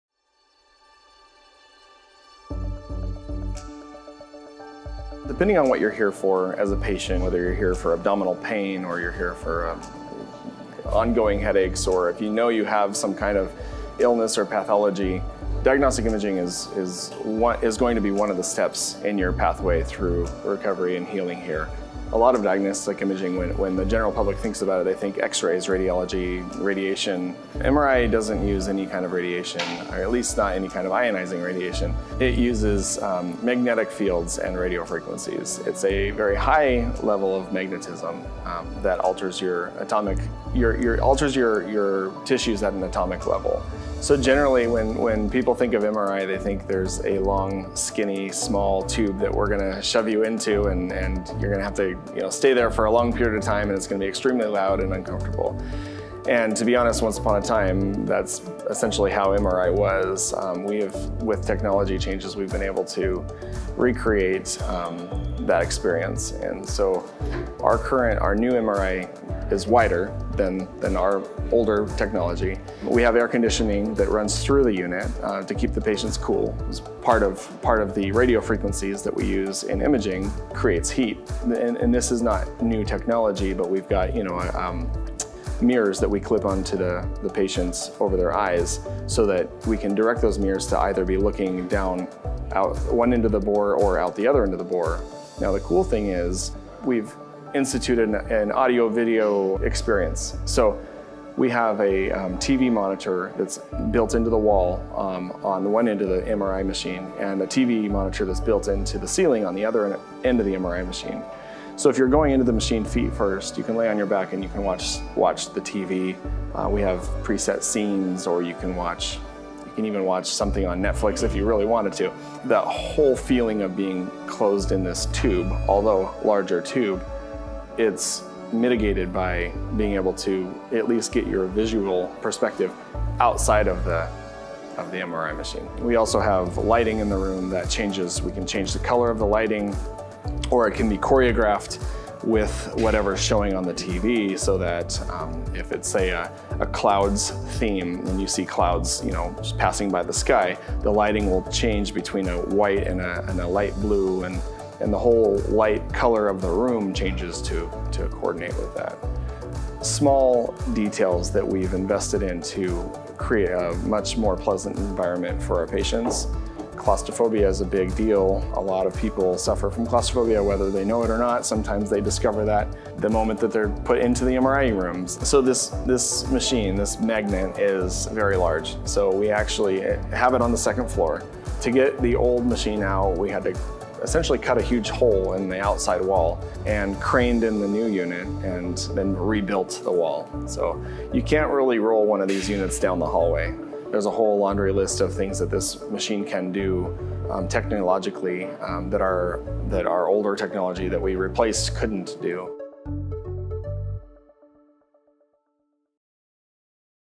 VIDEO: PeaceHealth Southwest’s MRI unit completes major upgrade
Recently, PeaceHealth Southwest Medical Center was able to install a brand new MRI machine to replace their current model which was installed in 1989 and upgraded many times over the years. The new machine comes in at a market value of more than $2 million alone.